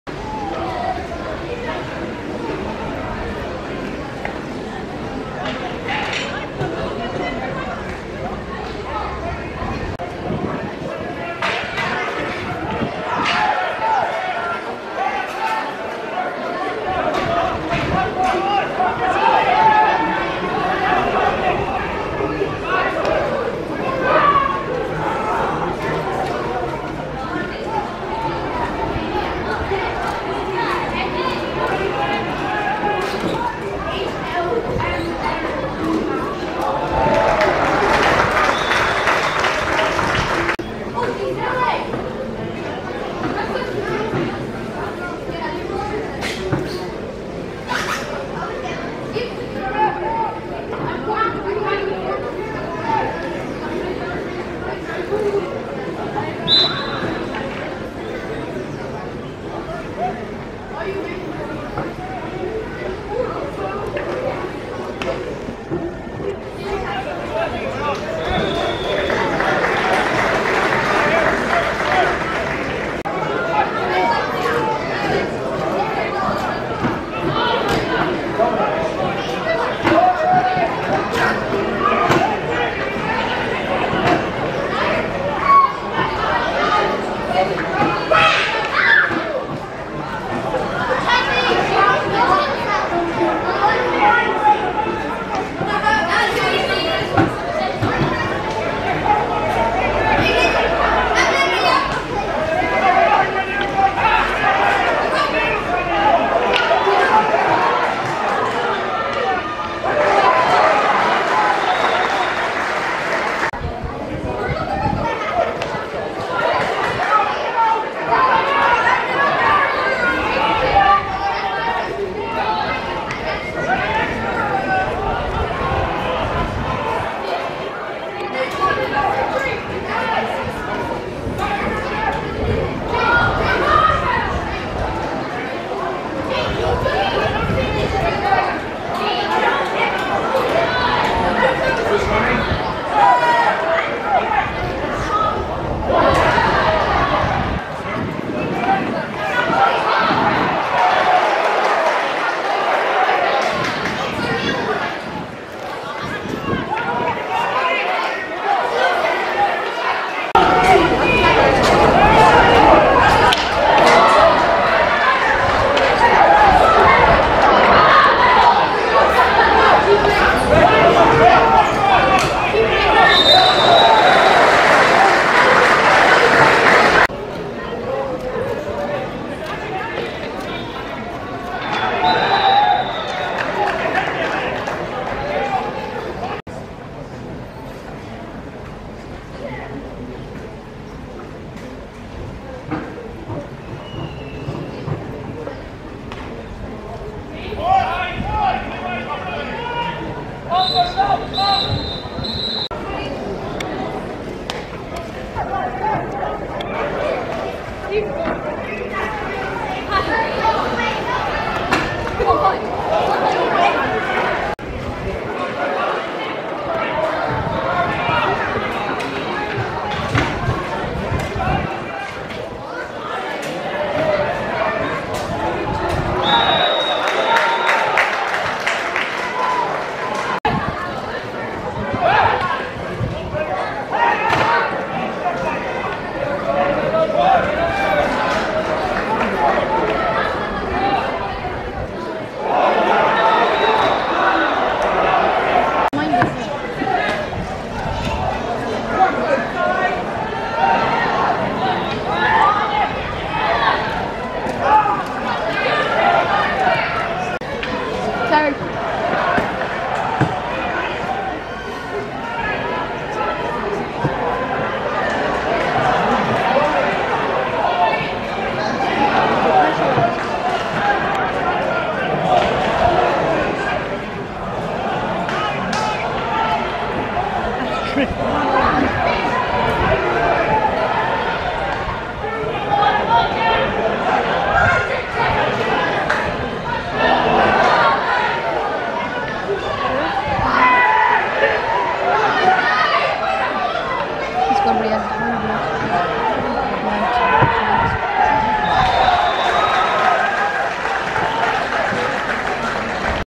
Castlegar V Liam Mellows Galway Senior Hurling Championship 2019
Galway Senior Hurling Championship 2019 in Pearse Stadium, Galway, Castlegar V Liam Mellows.